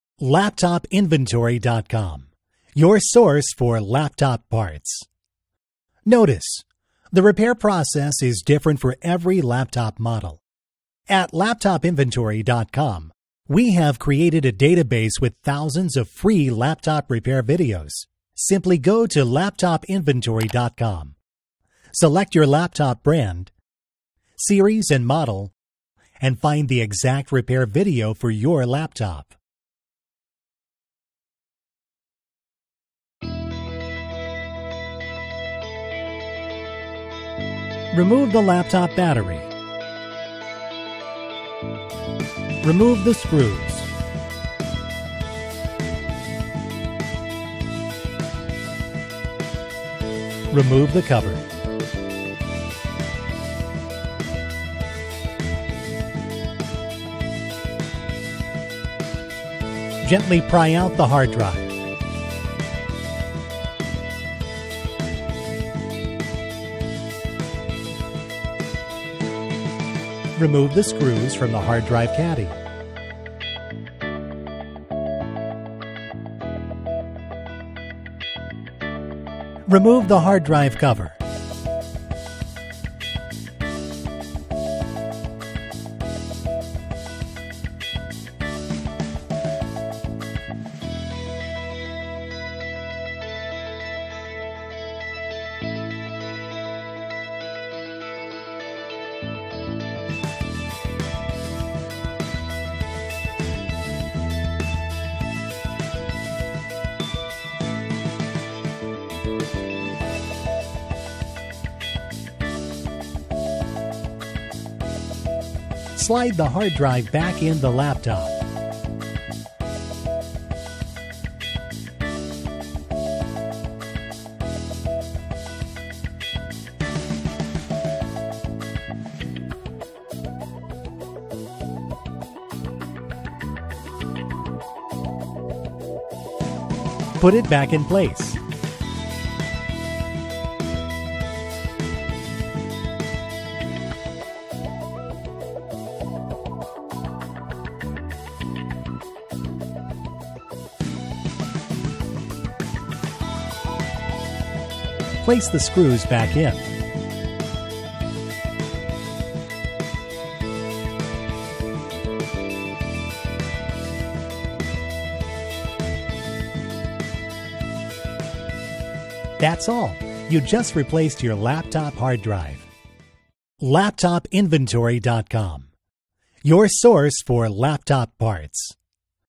HP DV6-3000 Hard Drive Replacement - Pavilion Laptop Install Repair Guide
: HP DV6-3000 Hard Drive Replacement - Pavilion Laptop Install Repair Guide - DV6-3000 DV6-3033TX DV6-3033HE DV6-3034 DV6T-3000 DV6Z-3000 DV6-3100 DV6-3034NR DV6-3034TX DV6-3035 DV6t-3100 DV6t-3200 DV6t-4000